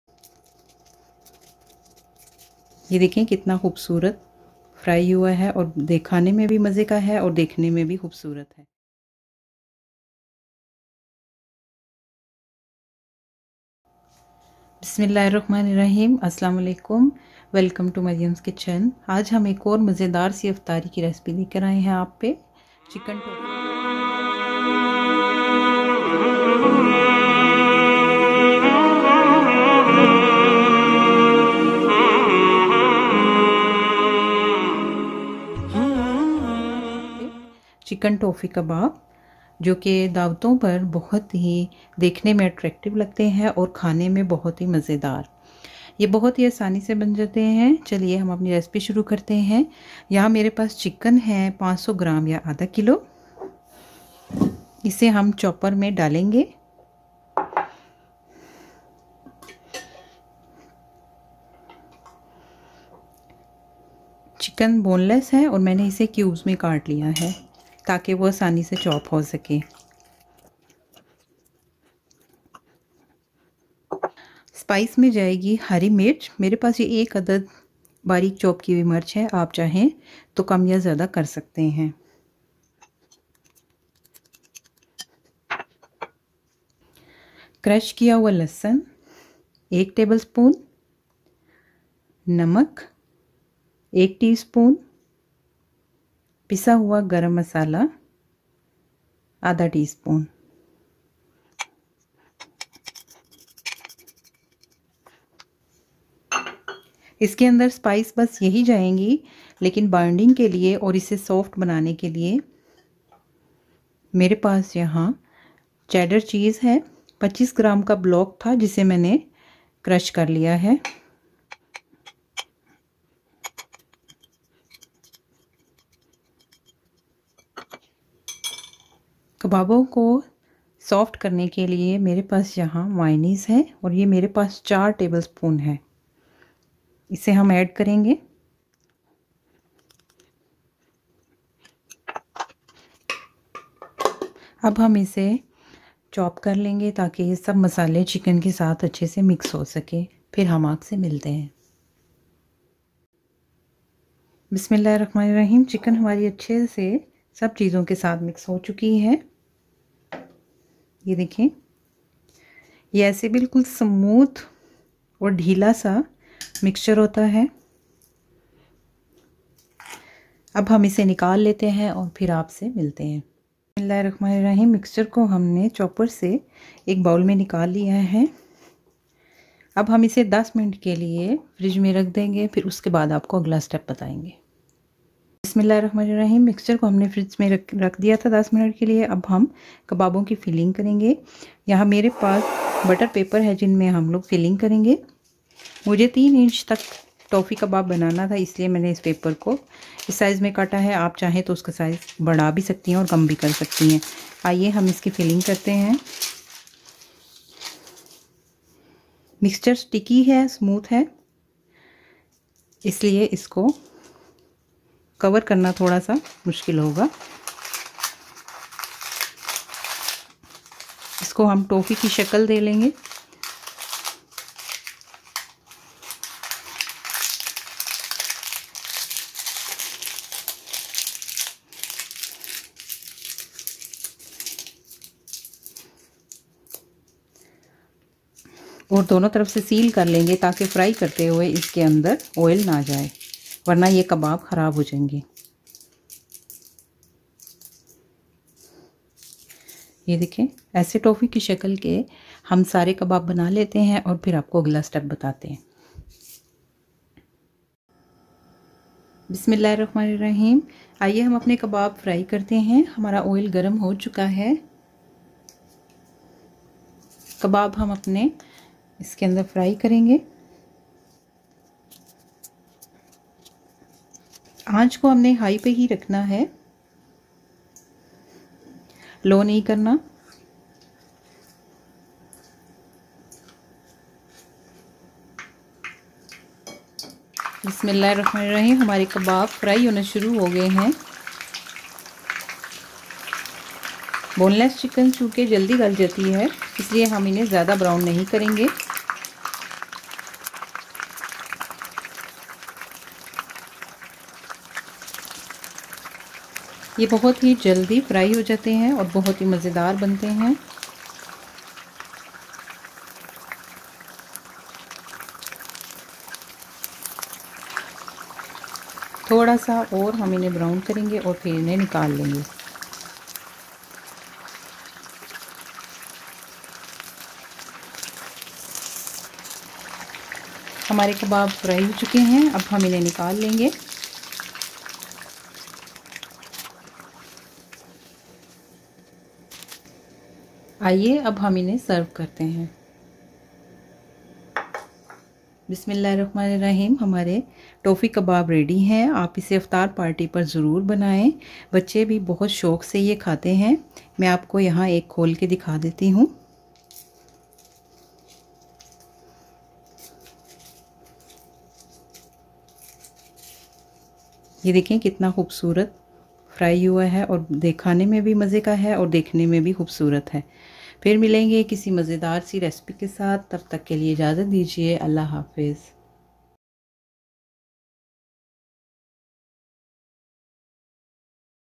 Toffee Kabab | Butter Paper Kabab | Chicken Cheese kebab | by Maryam's kitchen - باورچی خانہ 🍗🍢🍬
How to make chicken Toffee kabab
Butter paper kabab
Chicken Cheese kebab

aj hum bna rahy han CHICKEN TOFFE KABAB  yan BUTTER PAPER KABAB . yeh boht asani sa ban jaty han or boht hi mazydar tyar hotay han. ap bhi bananin apni dawtoon man yakenan apka mehmano ko bhi boht pasand aingay .